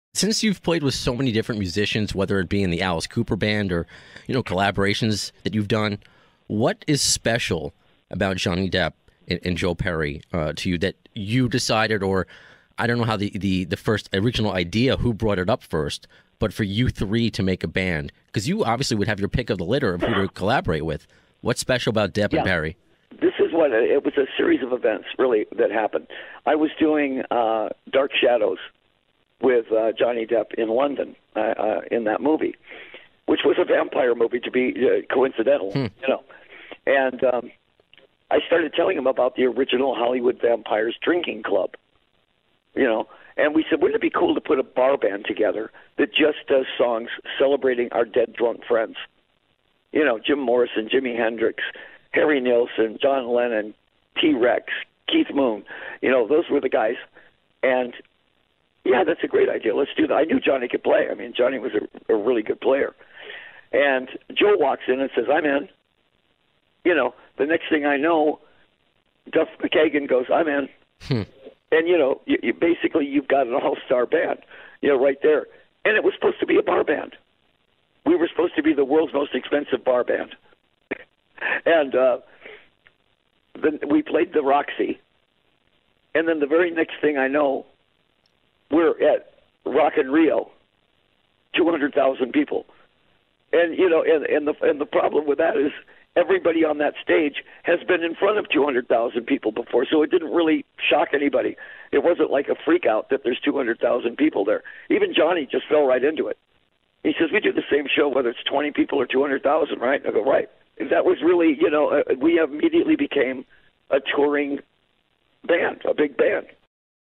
Alice Cooper talks Hollywood Vampires | AFD CLIPS
- Recorded 6-26-19 -

Alice Cooper talks about how he formed the Hollywood Vampires with Johnny Depp, Joe Perry, and Duff McKagan. Full ep

Pics via Getty